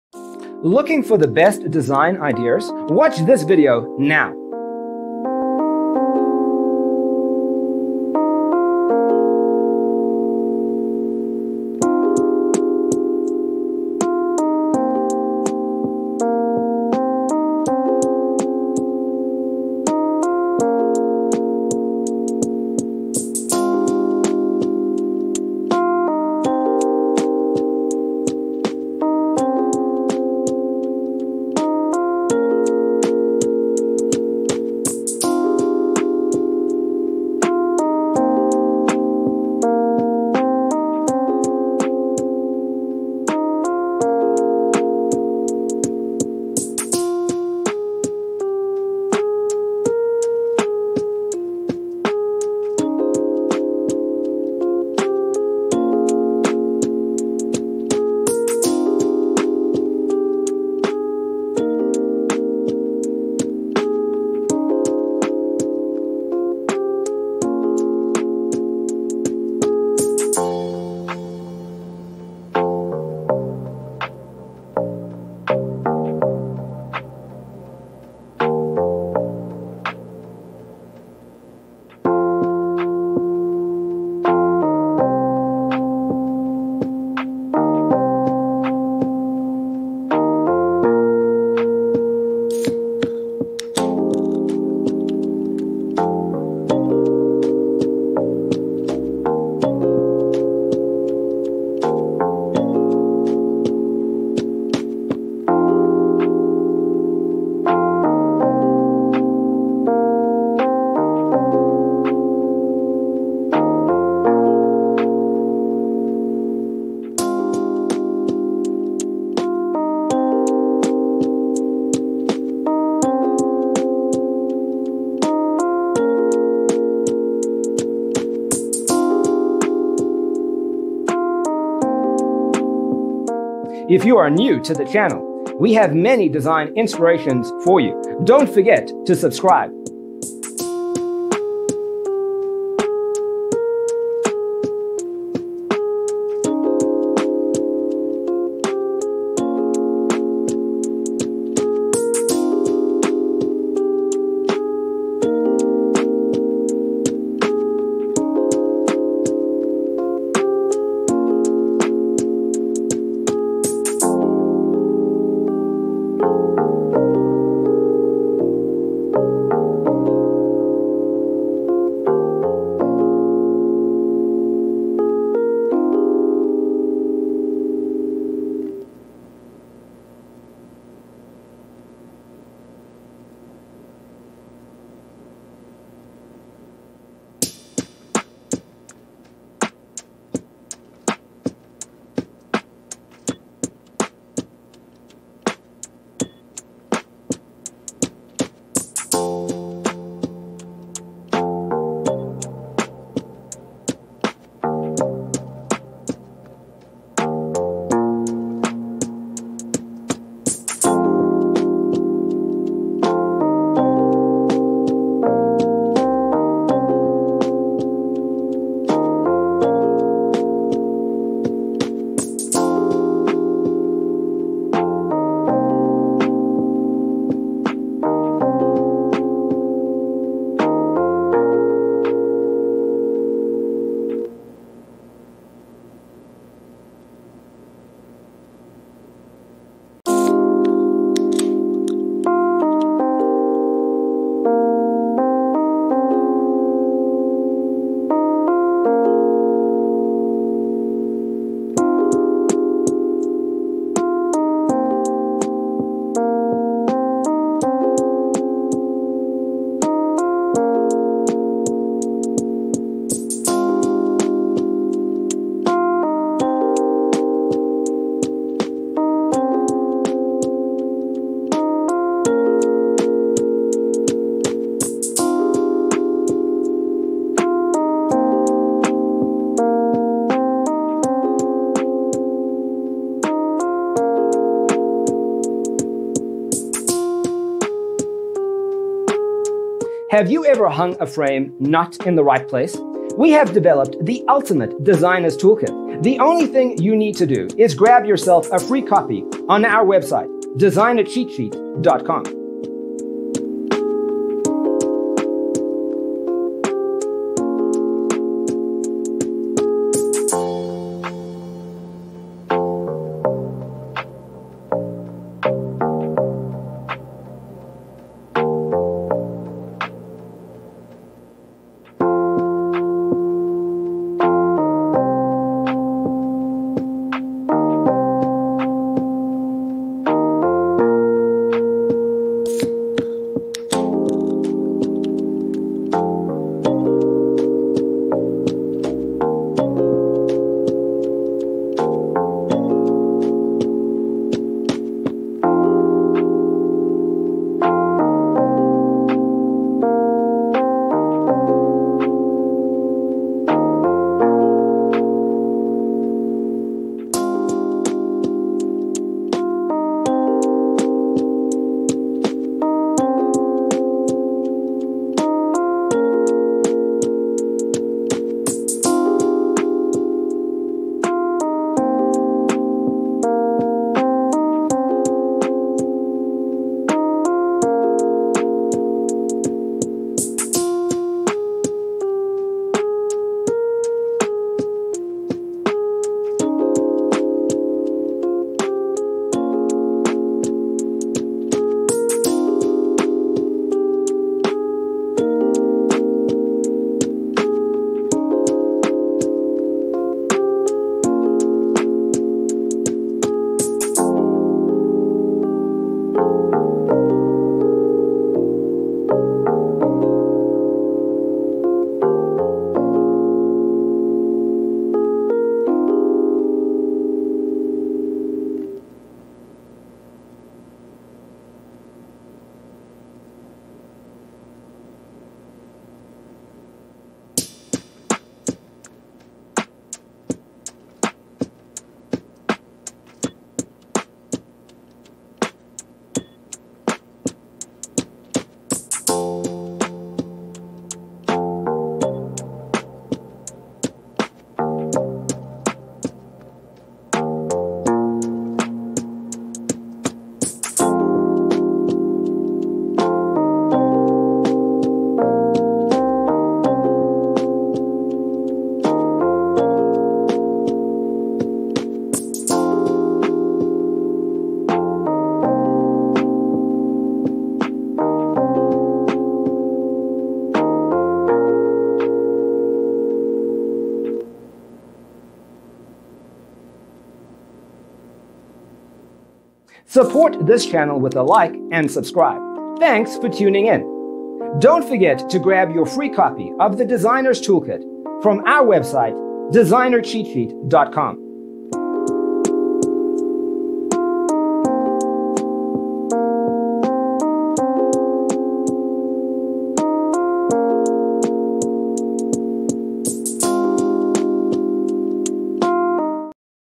75 Huge Modern Kitchen Design Ideas You'll Love ☆
Browse photos of Huge modern kitchen designs. Discover inspiration for your Huge modern kitchen remodel or upgrade with ideas for storage, organization, layout and decor.

0:00:00 - Intro
0:00:12 - Huge Modern Kitchen Design Ideas You'll Love: Inspirations 1 to 25
0:02:56 - Huge Modern Kitchen Design Ideas You'll Love: Inspirations 26 to 50
0:05:53 - Huge Modern Kitchen Design Ideas You'll Love: Inspirations 51 to 75